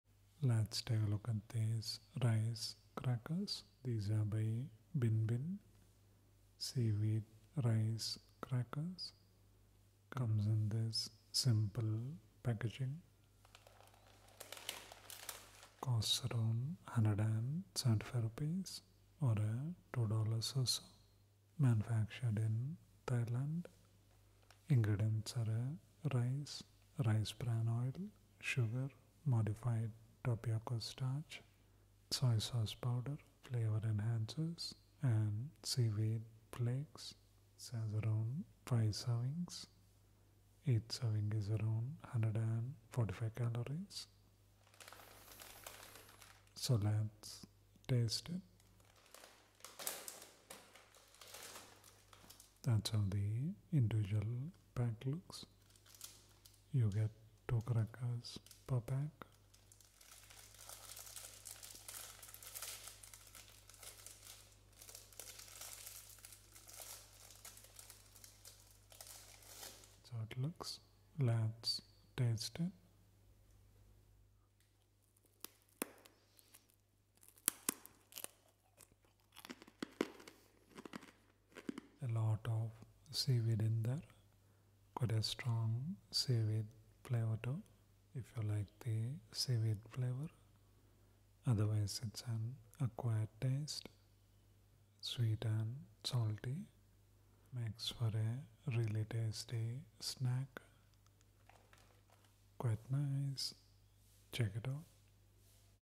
Bin Bin Seaweed Rice Crackers Asmr
These Bin Bin Seaweed Rice Crackers are quite crunchy and delicious. It costs ₹175 around $2.